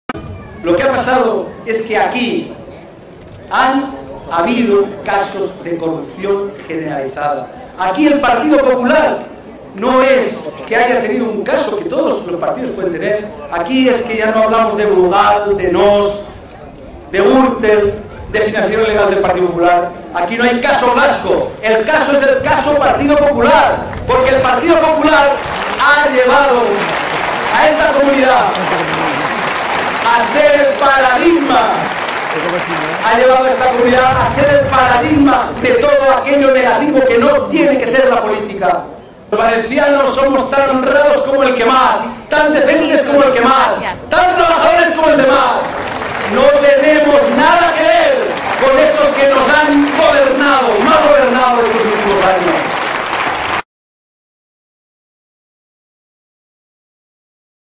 Ximo Puig: "No es el caso Blasco o el caso Nóos, es el caso Partido Popular"
El secretario general del PSPV-PSOE, Ximo Puig, ha asegurado que ha asegurado que "los valencianos estamos hartos de tanta corrupción y no merecemos ser ejemplo en toda España de lo peor de la política", al tiempo que ha acusado a Fabra y al PP "de manchar el buen nombre de la Comunitat". En este sentido, ha advertido de que "no se trata del Caso Blasco, ni el caso Gürtel, ni el caso Noos o el caso Brugal, se trata del caso PP, de la madre de todas las corrupciones".